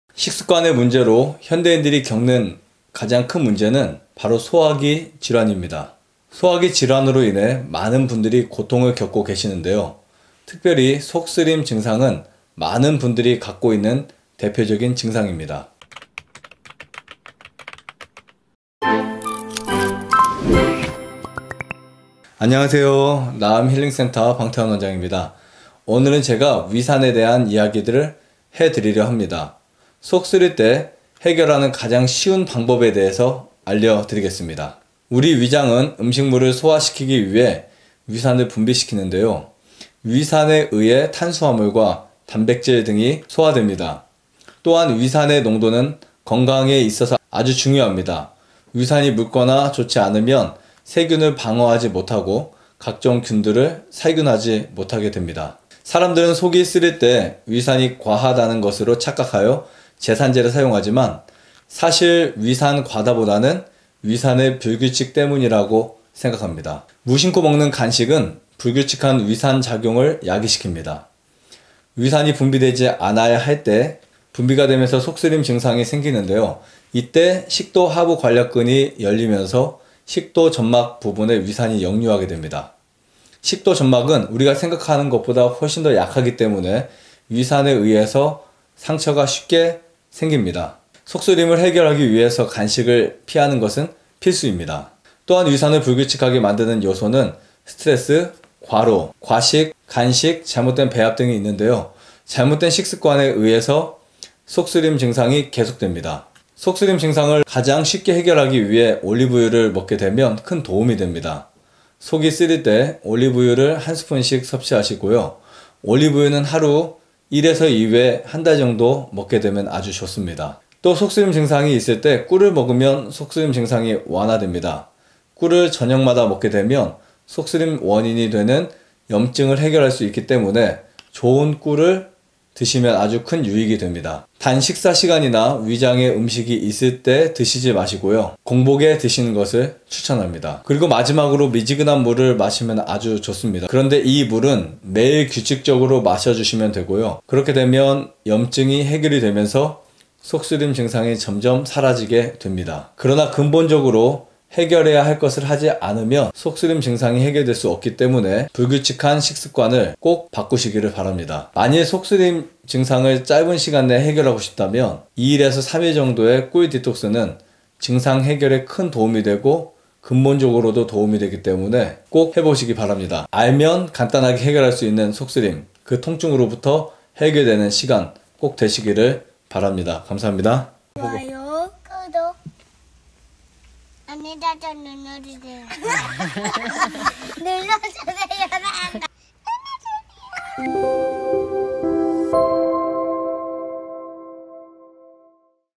속쓰림을 해결하는 가장 쉬운 방법 [방태환 원장의 5분 건강정보]
속 쓰림 증상을 간단하게 해결 할 수 있다?